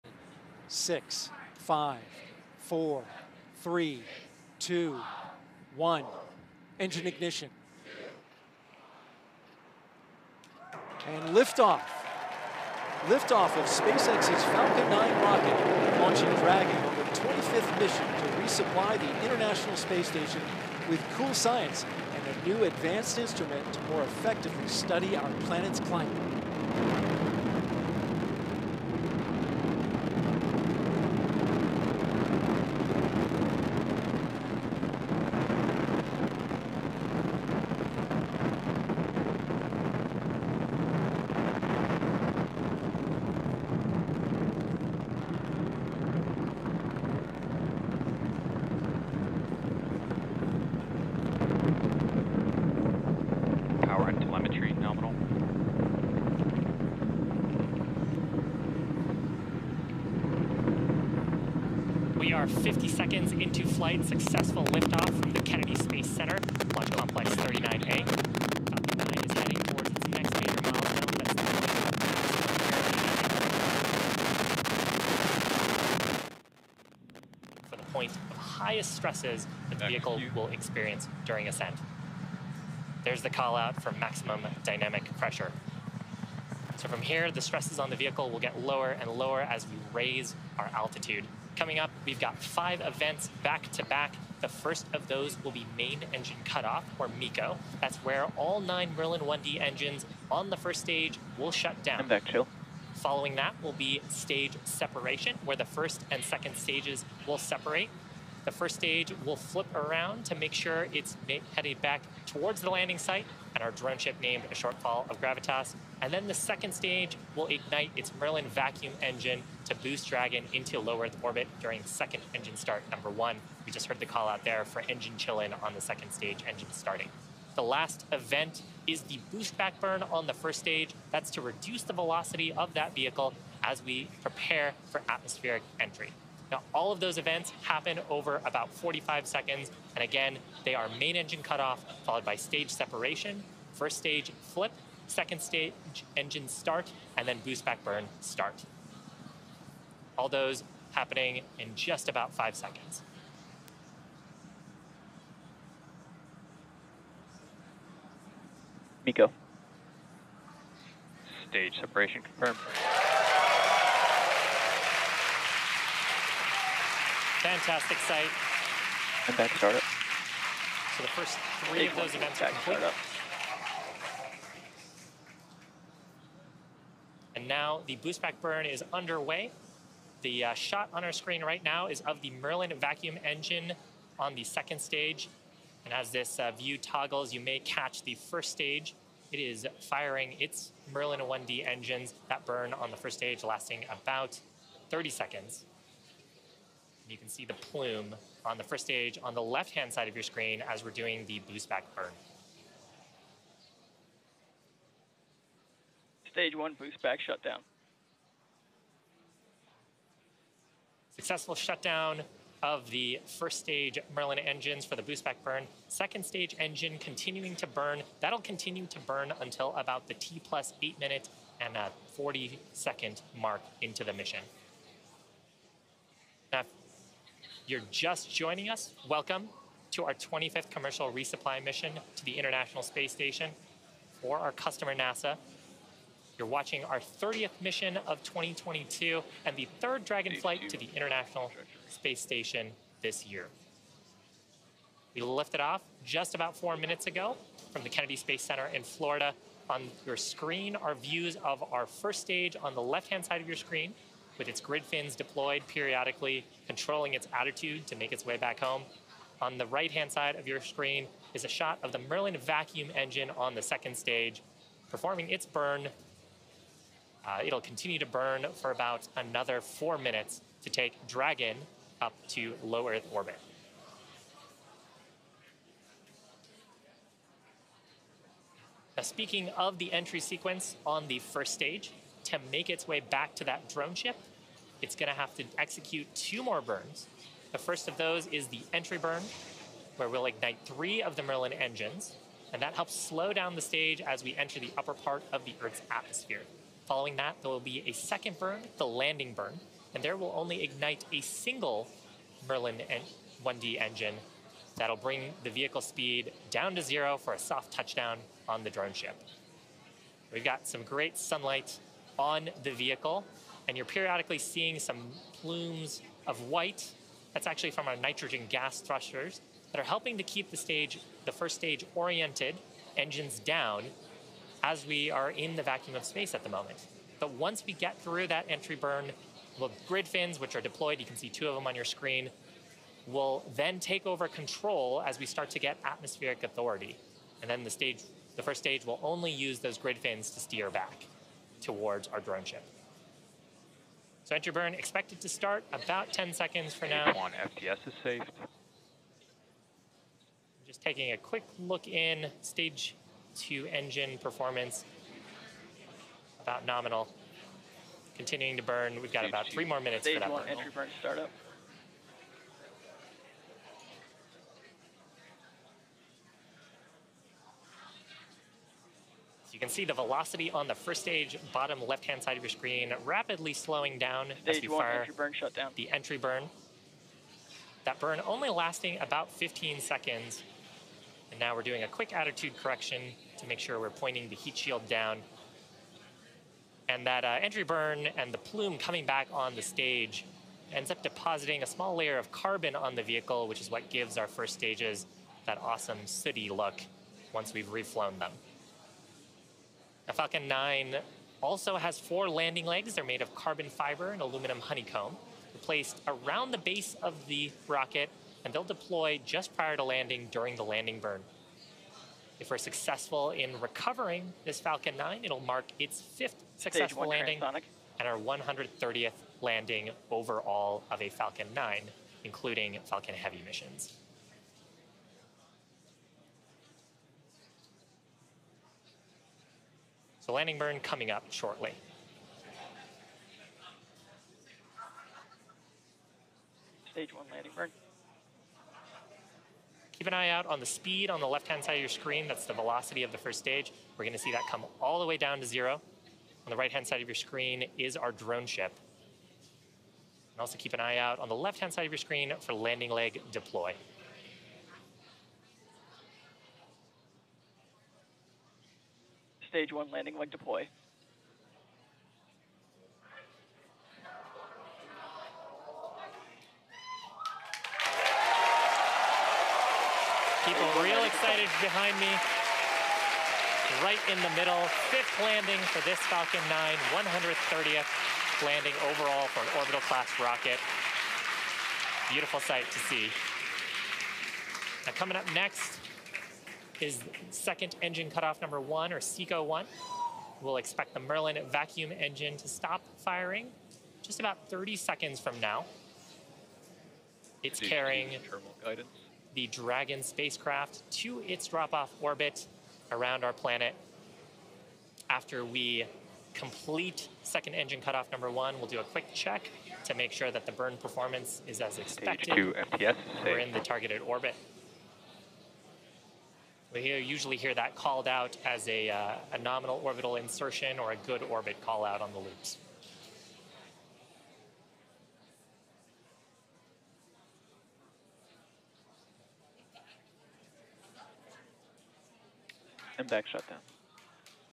SpaceX launches CRS-25 Cargo Dragon mission to space station, nails landing
SpaceX launched the Commercial Resupply Services-25 (CRS-25) mission to the International Space Station from Launch Complex 39A (LC-39A) at Kennedy Space Center in Florida on July 14, 2022. The first stage of the Falcon 9 rocket landed on the "A Shortfall of Gravitas" drone ship minutes later in the Atlantic Ocean. Full Story

Credit: SpaceX